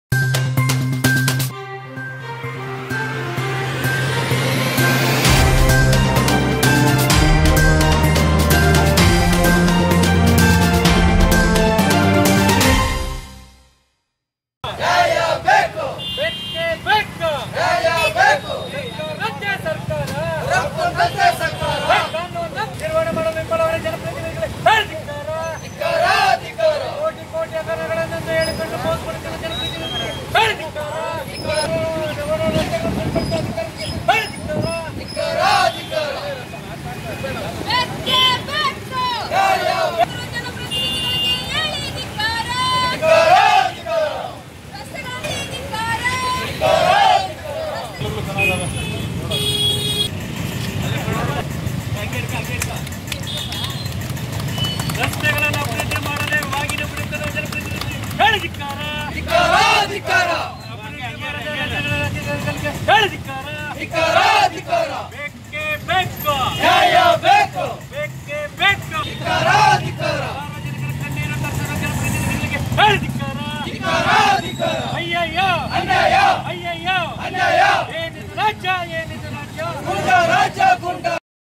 ಕೋಲಾರದಲ್ಲಿ ರೈತ ಸಂಘದಿಂದ ವಿನೂತನ ಪ್ರತಿಭಟನೆ.
ಕೋಲಾರ ನಗರದ ಟೇಕಲ್ ಸರ್ಕಲ್ ಬಳಿ ರಸ್ತೆ ತಡೆದು ಪ್ರತಿಭಟನೆ.

ಕೋಲಾರ ಶಾಸಕ, ಸಚಿವ, ಸಂಸದರ ಮುಖವಾಡಿ ಹಾಕಿಕೊಂಡು.

ರಸ್ತೆ ಮಧ್ಯೆ ಬಿದ್ದಿರುವ ಗುಂಡಿಯಲ್ಲಿ ನಿಂತ ನೀರಿನಲ್ಲಿ ಭಾಗನ ಅರ್ಪಿಸಿ ಪ್ರತಿಭಟನೆ.

ನಗರದಲ್ಲಿ ಬೃಹತ್ ಹೊಂಡಗಳಾಗಿ, ಕೆರೆಗಳಂತಾಗಿರುವ ರಸ್ತೆಗಳನ್ನ ಸರಿಪಡಿಸುವಂತೆ ಒತ್ತಾಯ.

ಇತ್ತೀಚೆಗೆ ಬಂದು ಮಳೆಯಿಂದ ರಸ್ತೆಗಳೆಲ್ಲಾ ಕೆರೆ ಕುಂಟೆಗಳಂತ್ತಾಗಿದ್ದು,
ಪ್ರತಿನಿತ್ಯ ವಾಹನ ಸವಾರರು, ಸಾರ್ವಜನಿಕರು ಪರದಾಡುತ್ತಿದ್ರು ಸರಿಪಡಿಸಿಲ್ಲ ಎಂದು ಅರೋಪ.